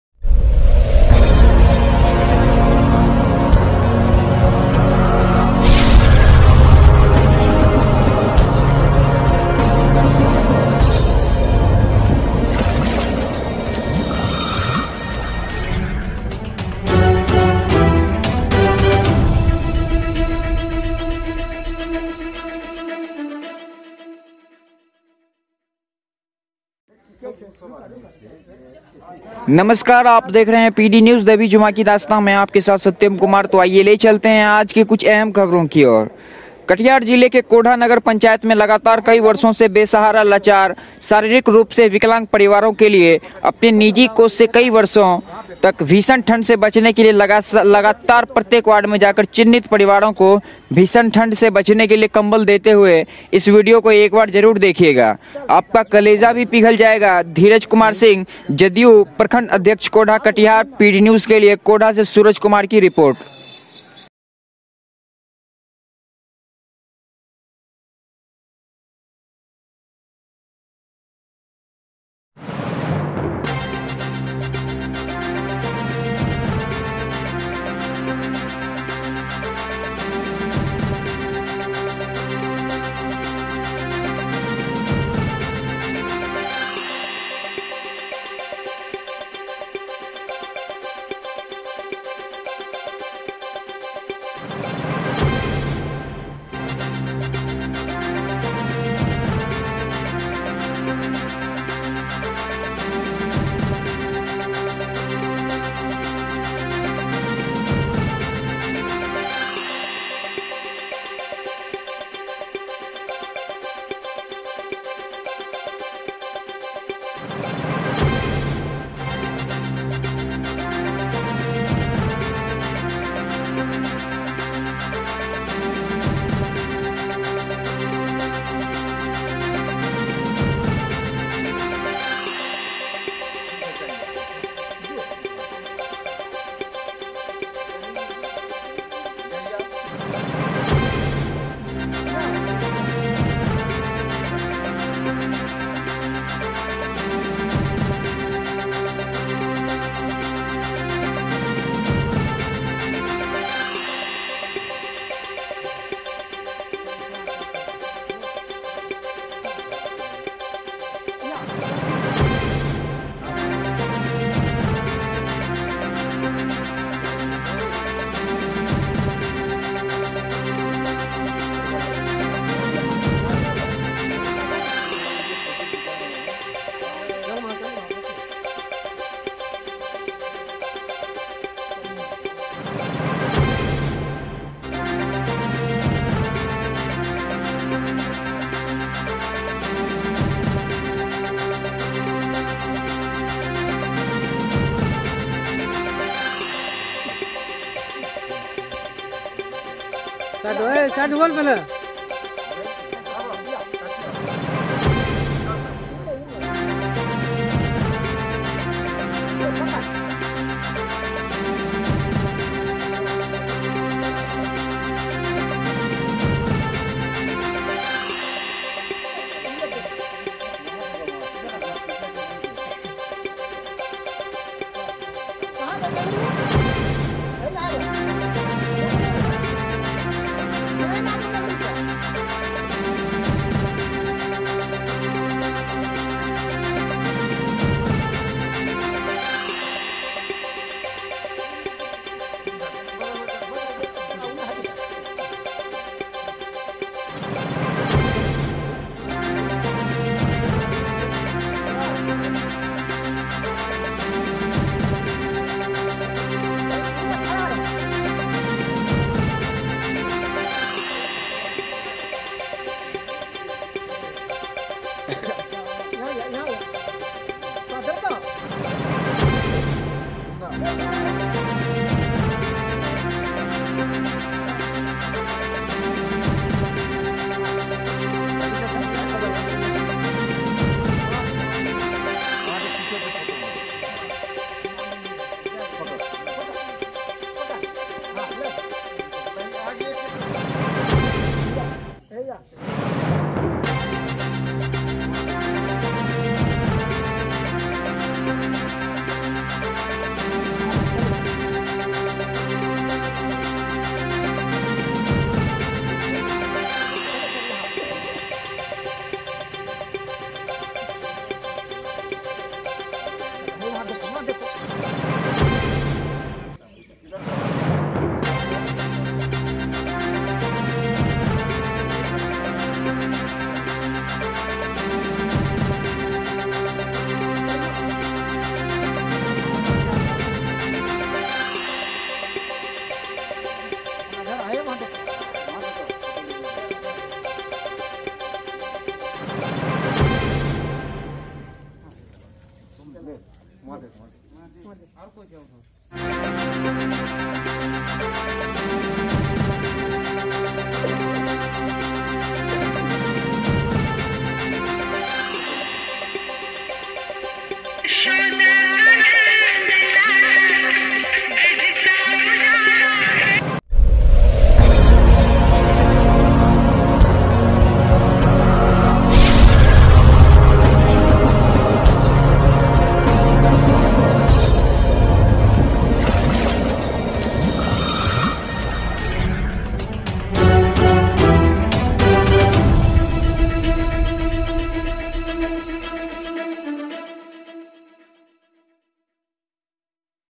कोढ़ा कटिहार# बेसहारा लाचार शारीरिक रूप से विकलांग परिवारों के बीच बांटा गया कंबल ।
PD News Bihar#PD News Delhi# कोढ़ा कटिहार# बेसहारा लाचार शारीरिक रूप से विकलांग परिवारों के बीच बांटा गया कंबल कोढ़ा से सूरज कुमार सिंह की रिपोर्ट ।